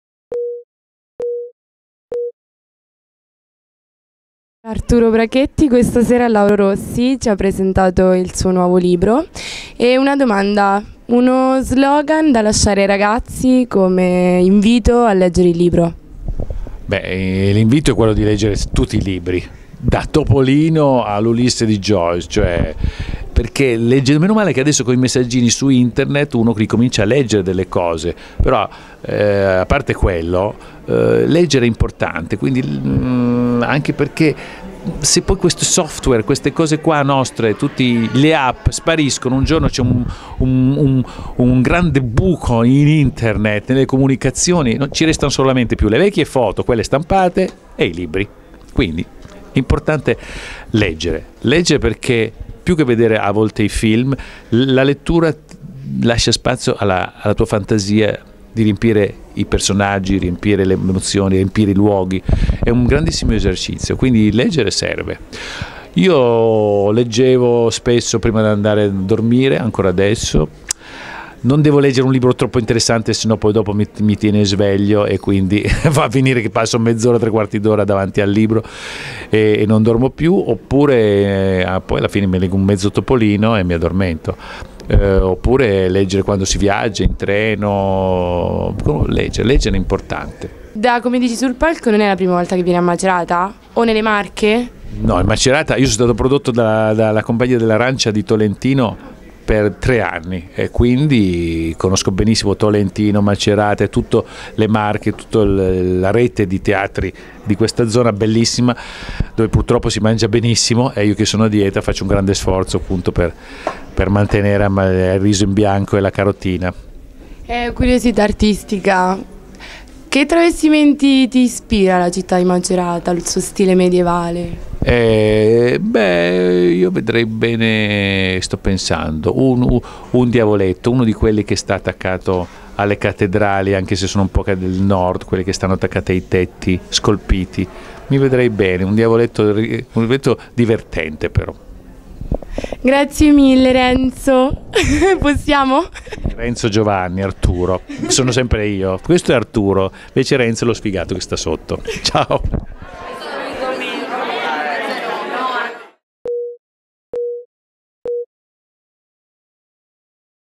Intervista Arturo Brachetti - TYCHE Magazine
nel corso di Macerata Racconta 2016 intervista di Arianna Cingolani